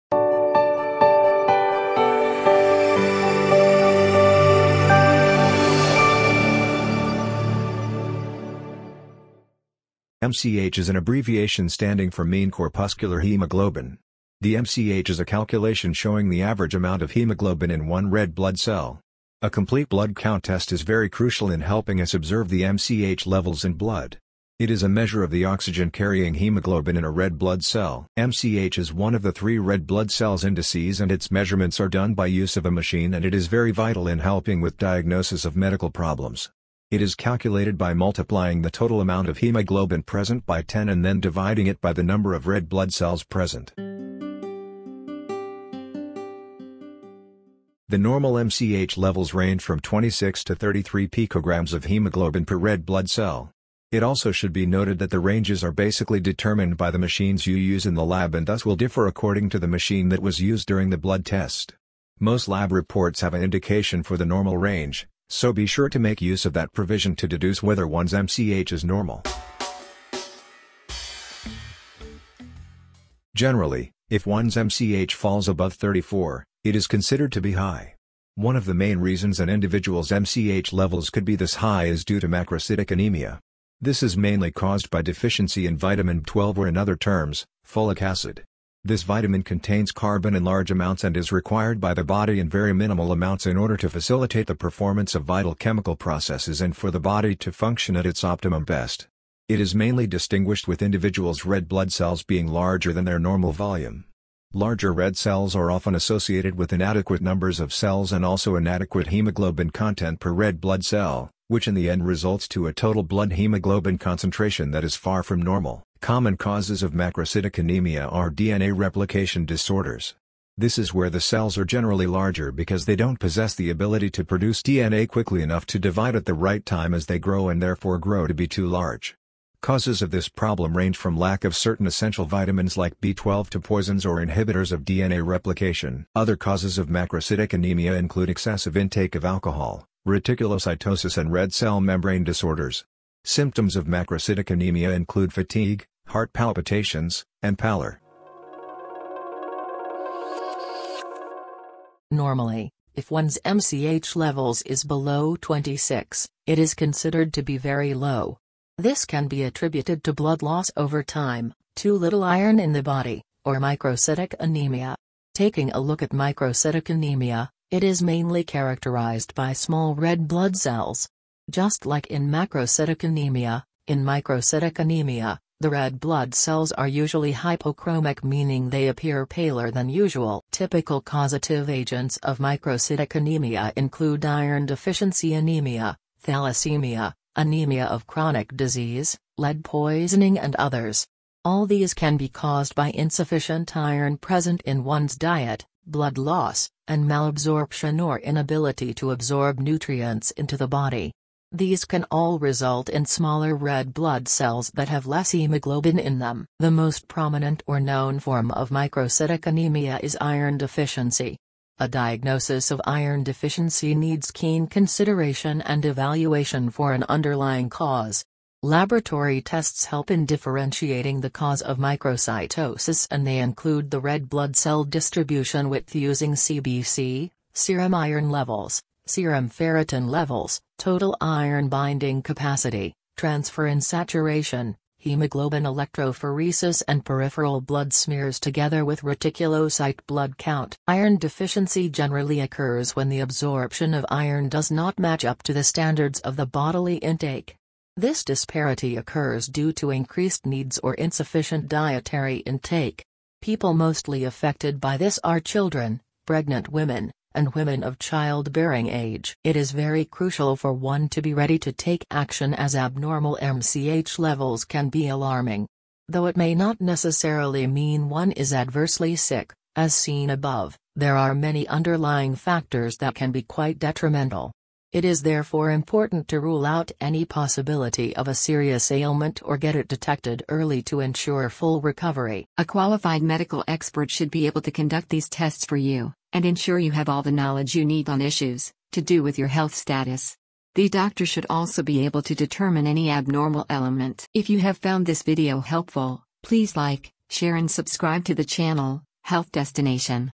MCH Blood Test – Low, High (Causes), Normal Levels
What is MCH in a blood test? Explanation of what it means, causes for levels to become high, low and the normal range
Easy Blood Typing kit for testing at home